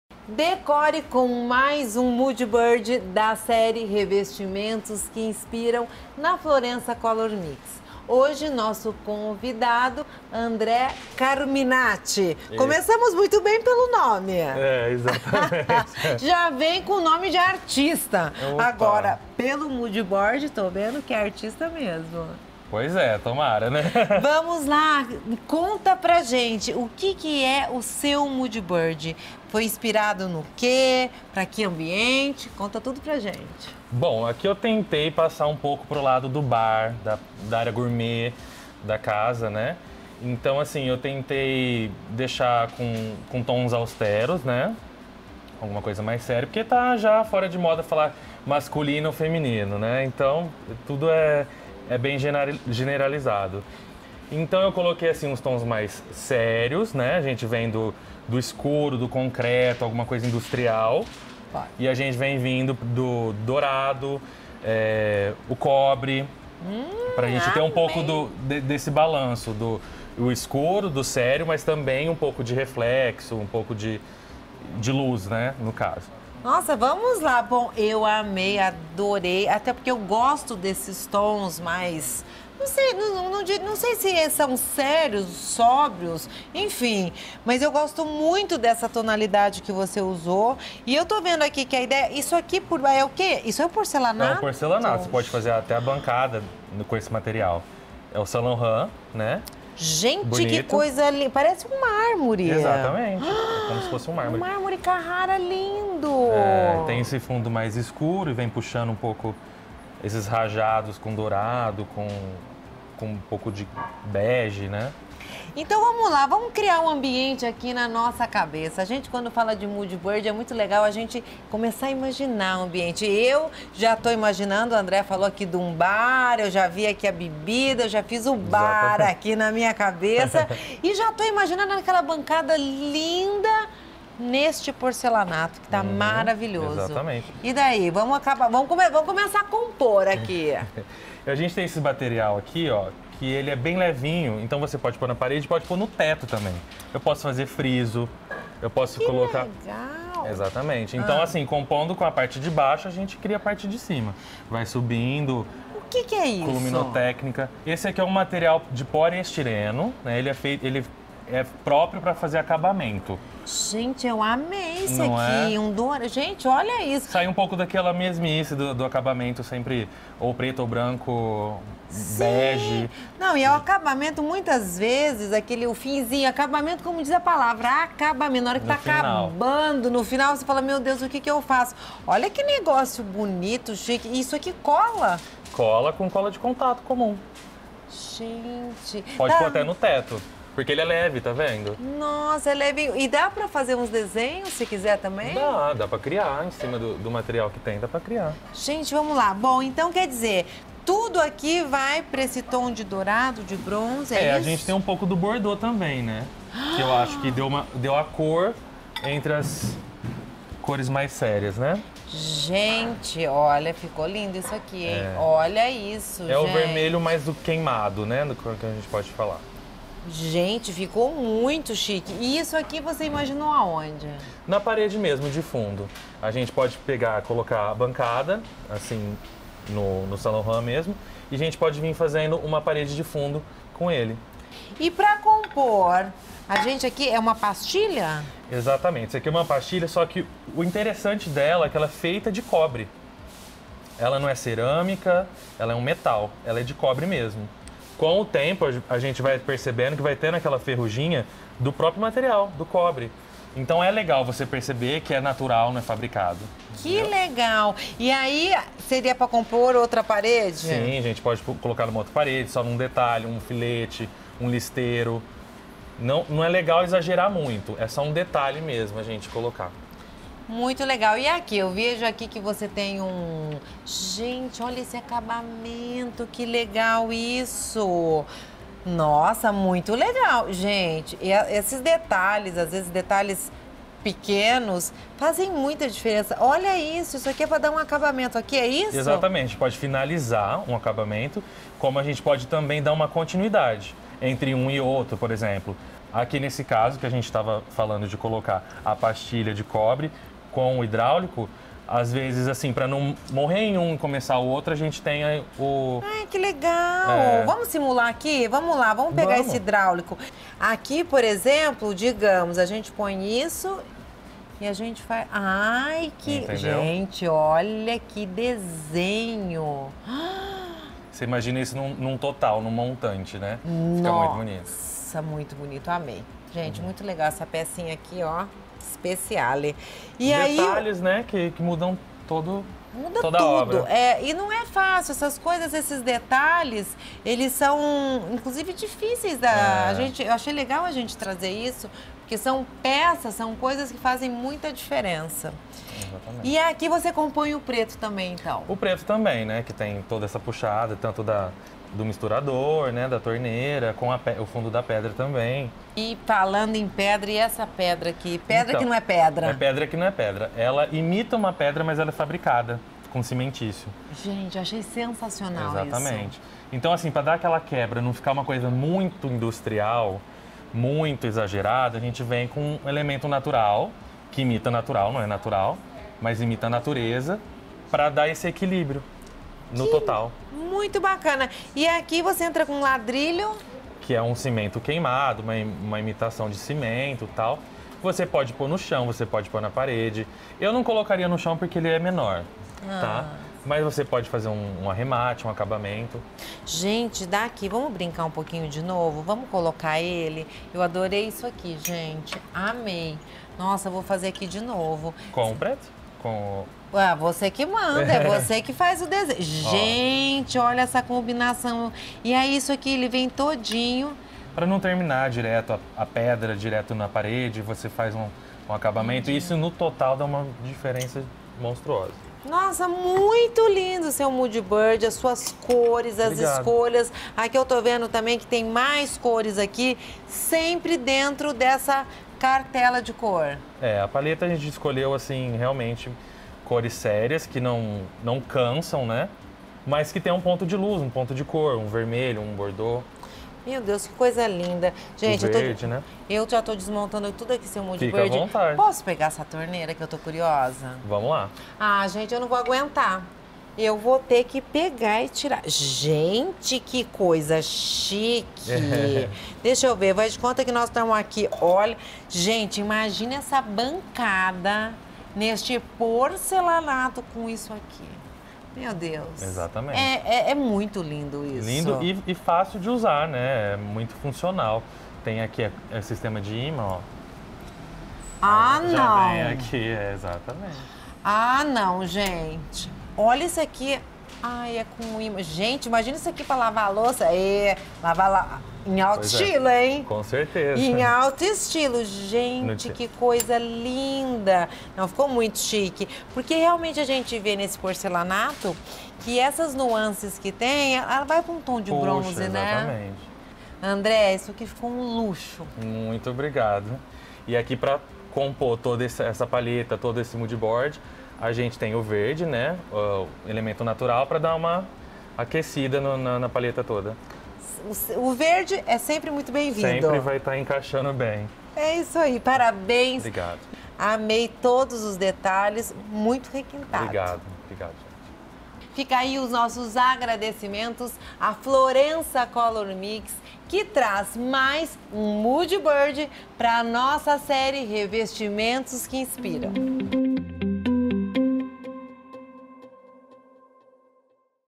COMO ESCOLHER REVESTIMENTOS PARA UM HOME-BAR? | LELINHA GENTIL
Na Florença Color Mix, André Caminati da Stanzza, fez um moodboard com uma combinação de ideias de tons austeros em contraste com o bordô para um home-bar de respeito.